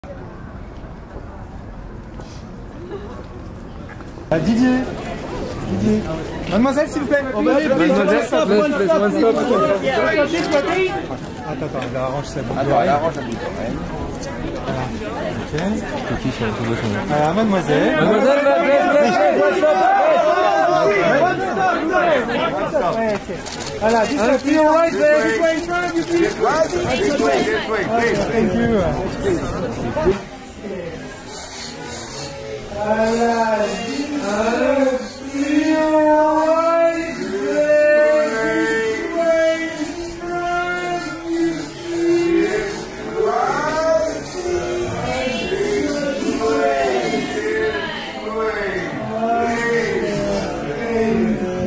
Lucy Boynton at the Chloe Fashion show in Paris
We spotted Lucy Boynton at the Chloe Womenswear Fall/Winter 2022/2023 Fashion show.

Thursday 3rd, March 2022 - Paris, France